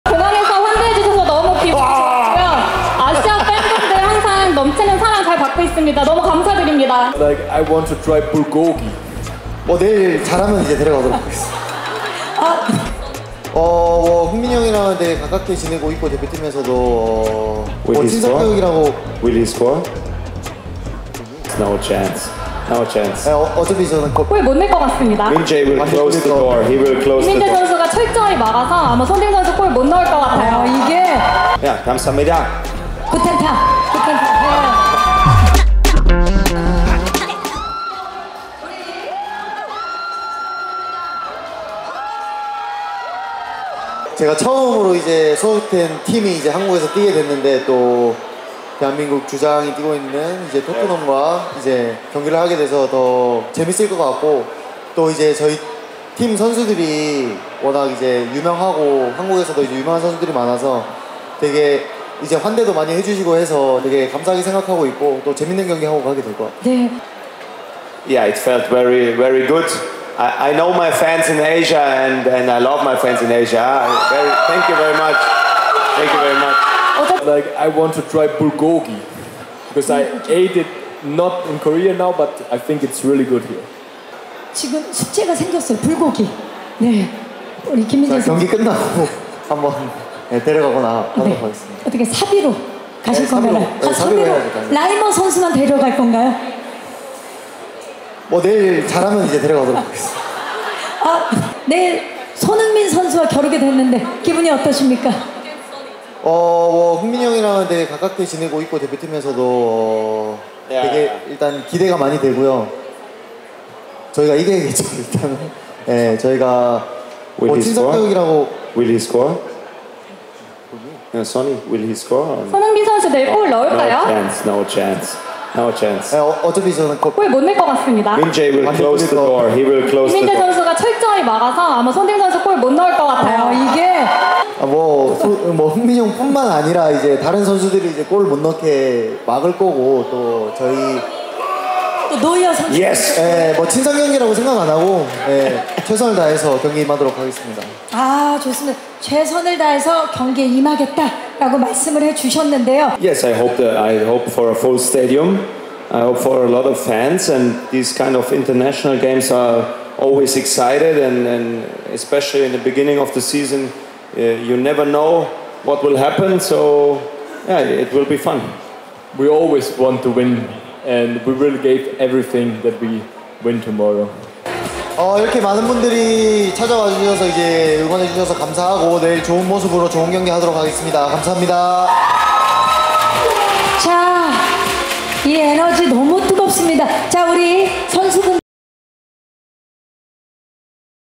"손흥민, 김민재가 다 막는다" 토트넘 빅매치 앞둔 바이에른 뮌헨 포부
(서울=뉴스1) 정윤경 기자,유수연 기자 = "그(손흥민)가 내일 골을 넣을까요? (골) 기회는 없습니다. 김민재 선수가 철저하게 막아서 손흥민 선수는 골 못 넣을 것 같아요."

한국을 찾은 독일 프로축구 분데스리가의 바이에른 뮌헨의 토마스 뮐러는  토트넘과의 맞대결을 앞두고 2일 이같이 말했습니다.

이날 오후 서울 여의도 IFC몰에서 열린 '아우디 써머투어 2024'에서 바이에른 뮌헨의 김민재, 토마스 뮐러, 콘라트 라이머는 팬들을 만났는데요.

3일 손흥민 선수가 소속된 토트넘 홋스퍼와 경기를 앞둔 소감을 묻는 말에 김민재는 "흥민이 형과는 되게 가깝게 지내고 있다"고 말했는데요.

이어 "친선 경기라고 생각 안하고 최선을 다해 경기에 임하겠다"고 하자 듣고 있던 토마스 뮐러는 "(손흥민에게) 기회는 없다"며 김민재가 다 막아줄 것이라고 말했습니다.